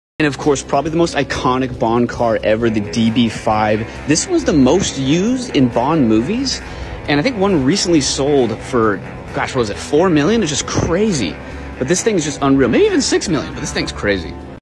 The best Bond car ever!
The Aston Martin DB5 used in a ton off bond movies!!